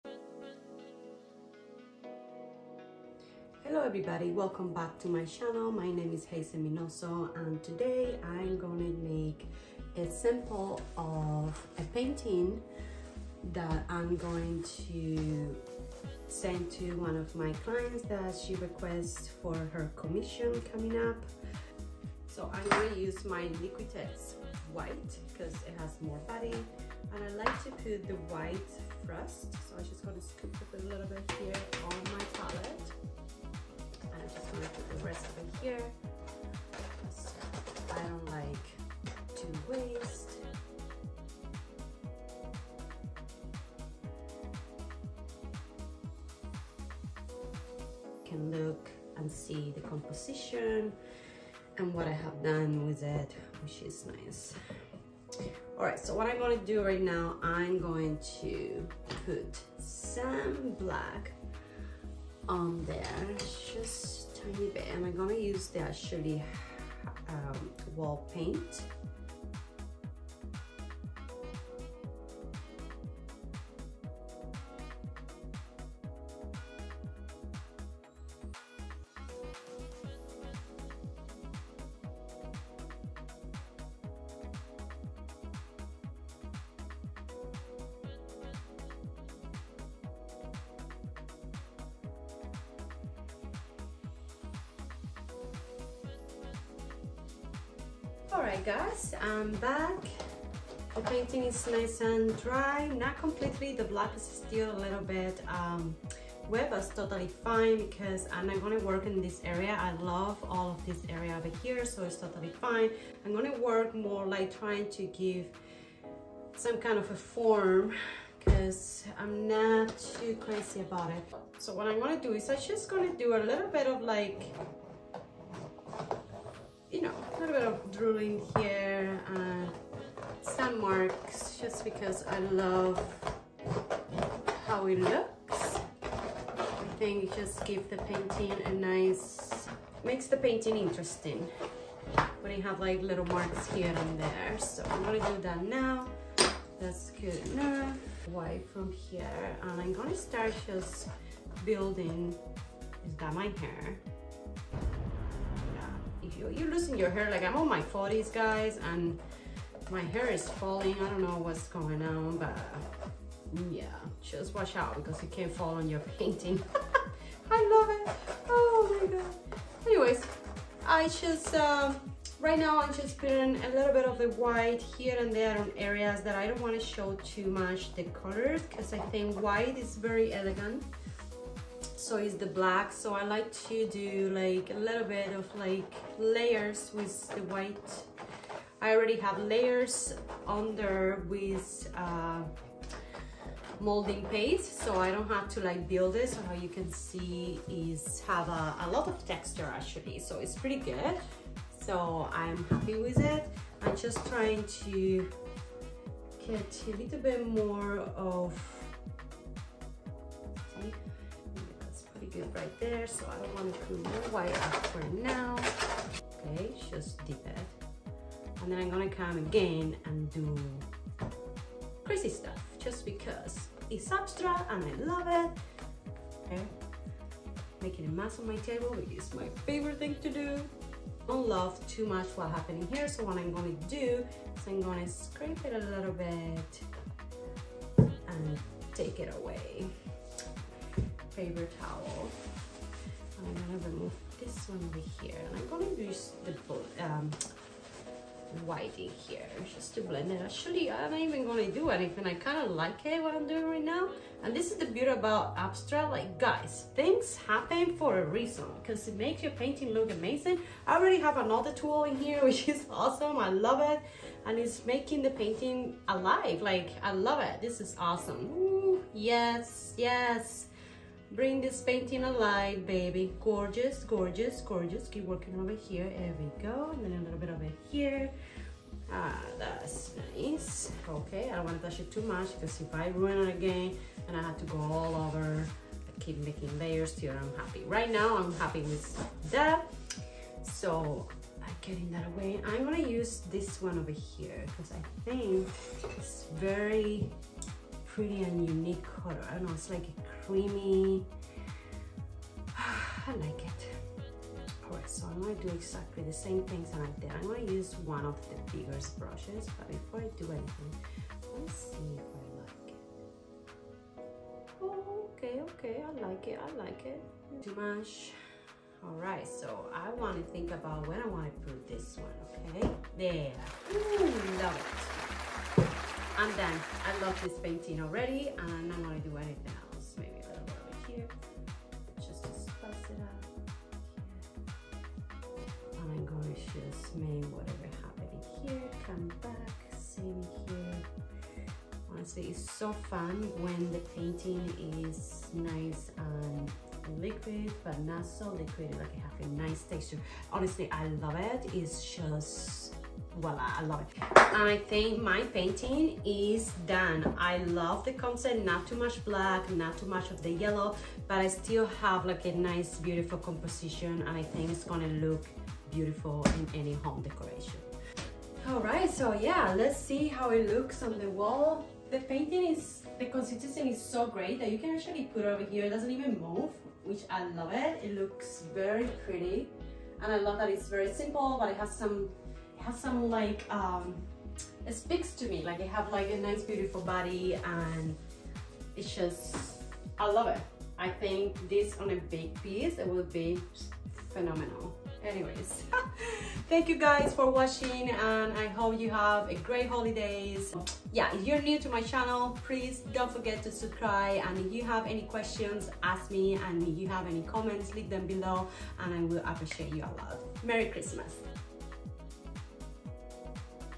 HOUSE PAINT abstract tutorial in my STUDIO.
I used house painting for this tutorial, love the consistency and brightness and also love how fast it dry. The colours i used here are from one of my clients samples of a commissioned piece, she shared them with me for me to get to a composition , style and color final before proceeding to the real painting .
I sometimes sale the samples , they attract many of my clients )
cheers and happy holidays.
Here are the Color’s I used :)

BM HC 9
BM HC 11

SW 9162
BM AF -550
BM 1665